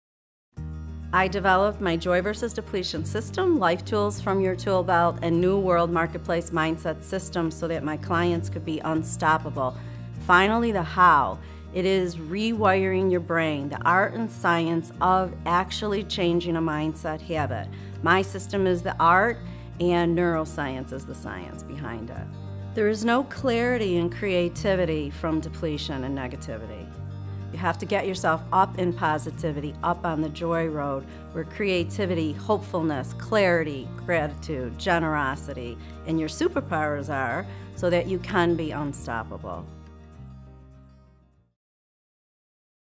Nancy Rizzo, Life and Mindset Wellness Coach
Nancy Rizzo, is a Lief and Mindset Wellness Coach who focus on re-shaping your mindset to embrace the gifts you have.